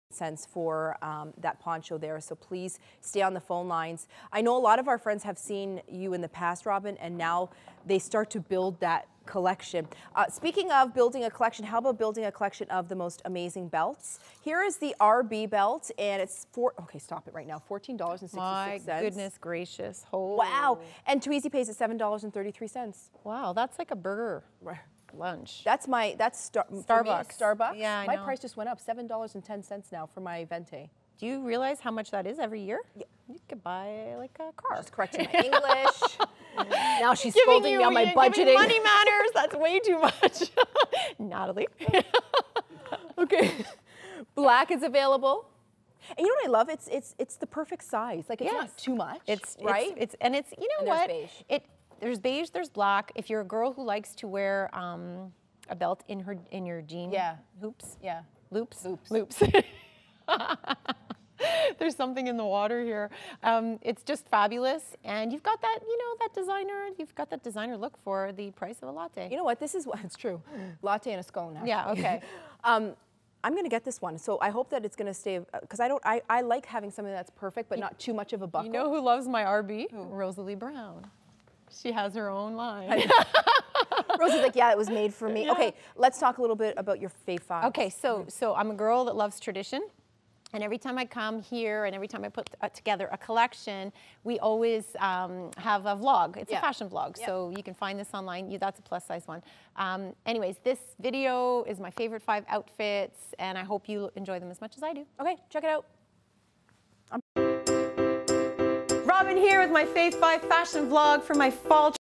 The RB Belt
Add a retro statement to your look with Robin Barre's monogram buckle belt, from Artizan!

• 'RB' silver belt buckle; (5) belt holes; textured finish
• Fabric: 100% polyurethane
• Care: do not wash; spot clean with damp cloth

Brand: Artizan by Robin Barre